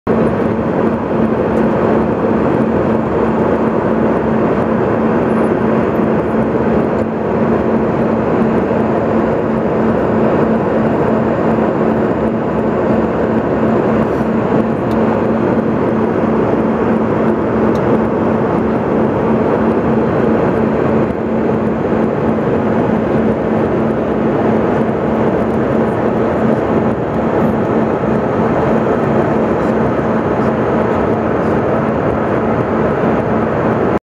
Plane flight into Bozeman, Monatana
i'd later learn that this is looking down on part of Yellowstone, everywhere you look is snow covered mountains